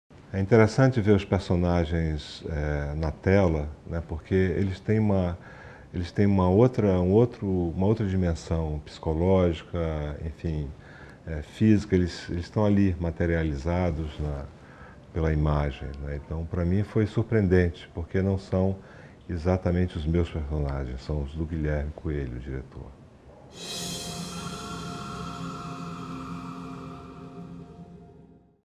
Milton Hatoum conta como é ver personagens de seu livro retratados no cinema
Escritor do romance que inspirou o longa metragem ÓRFÃOS DO ELDORADO fala da experiência de ver seus personagens retratados no cinema.